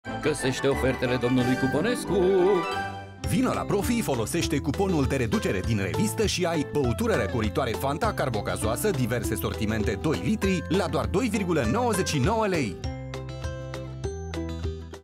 Foloseste cuponul de reducere din revista si ai Fanta diverse sortimente 2L la un pret special
Vino la PROFI, foloseste cuponul de reducere din revista si ai Băutură răcoritoare Fanta carbogazoasă diverse sortimente 2L la un pret special. Oferta supusa unor termene si conditii. Regulamentul campaniei pe Profi, zilnic preturi mici!